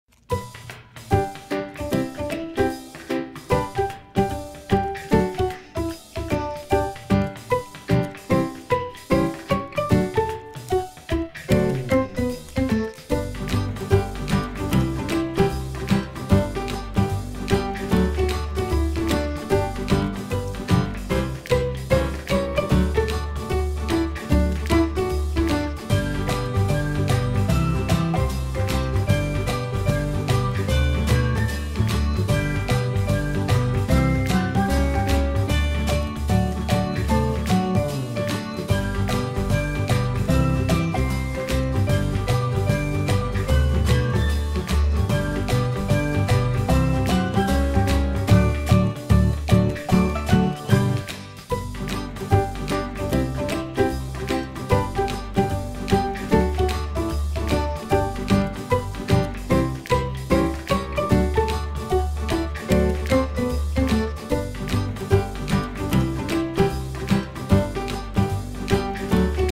Mae The Chipmunk (trying on her new hat!)
Mae is one of the Chipmunks I can regularly pet now, she let me set the tiny acorn hat on her cute little head!